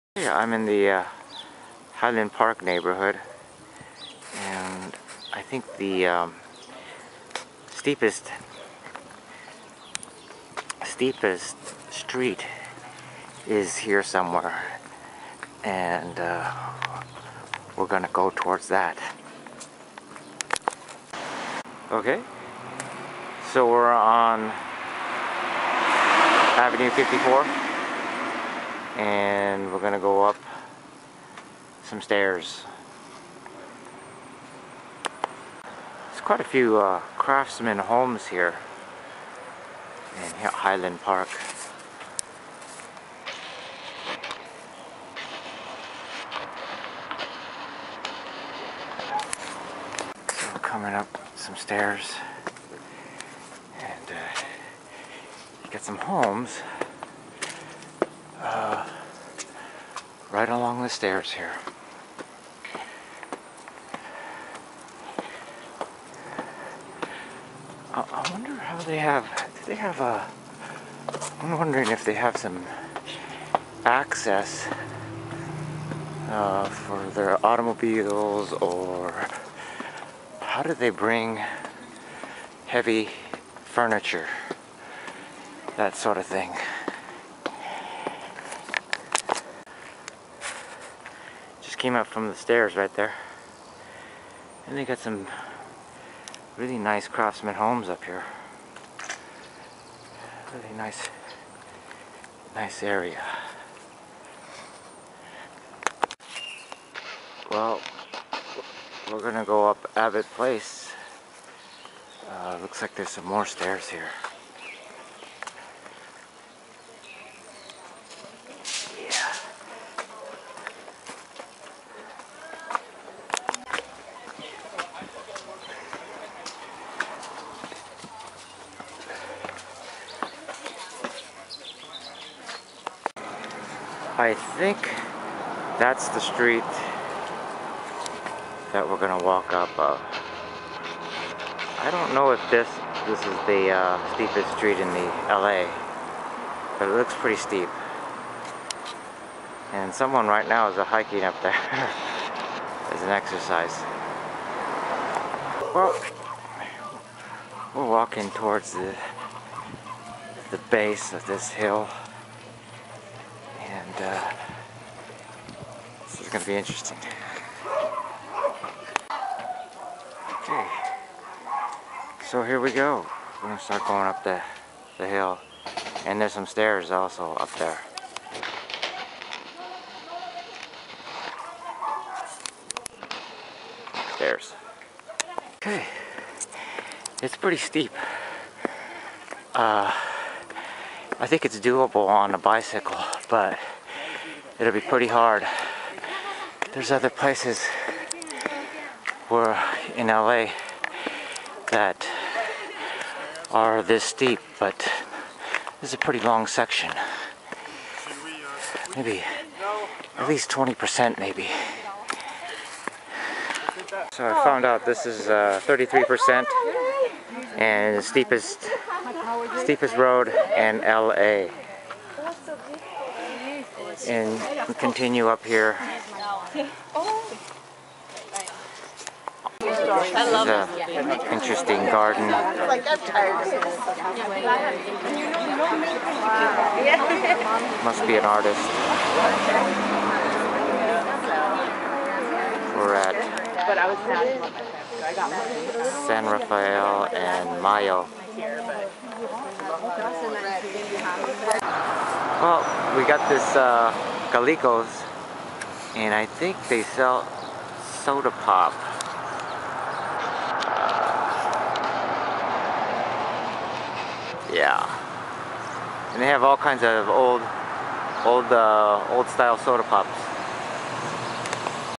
Highland Park and Mt Washington Eldred St steepest street in Los Angeles
I walked some residential streets in Highland Park and Mt. Washington neighborhoods of Los Angeles.  On Mt. Washington, I walked up Eldred street, which is the steepest street in Los Angeles at 33%.
Eldred was constructed in 1912, several decades before the city mandated that no streets have a grade steeper than 15%.

Recorded 18Jan15